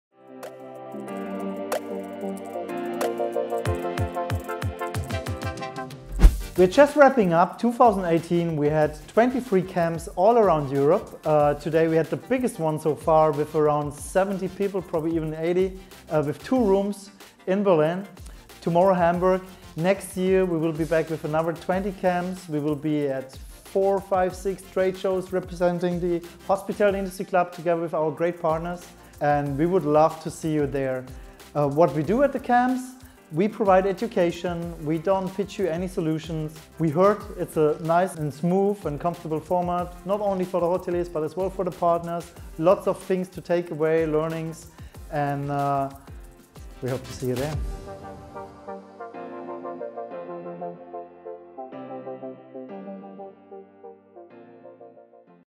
Hospitality Industry Club Preview 2019 - Daniel Zelling
Your host Daniel tells you what you can expect at the Hospitality Industry Club Events in 2019.